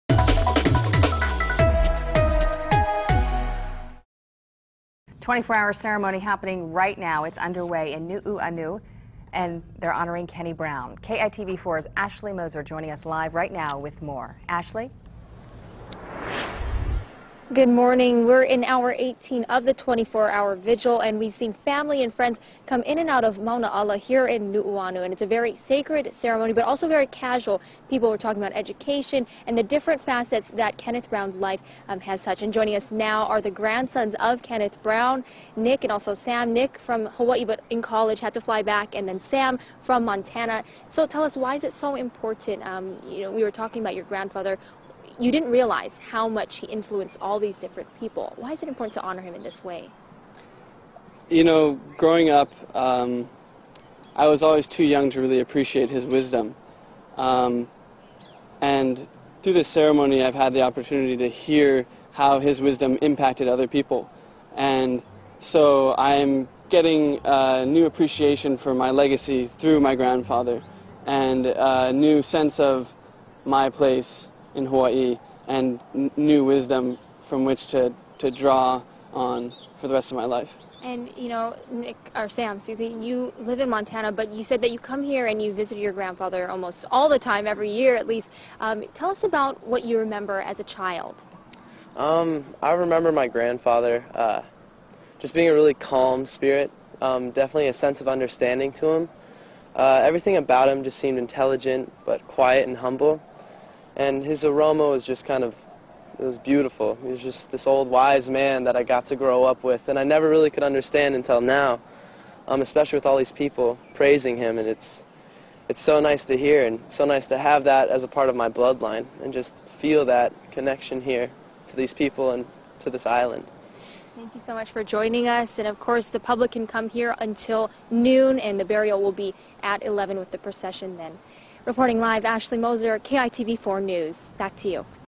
24-hour ceremony for Kenneth Brown underway
A 24-hour ceremony is underway in Nu'uanu honored the late Hawaiian leader Kenneth Brown. For news you need to know,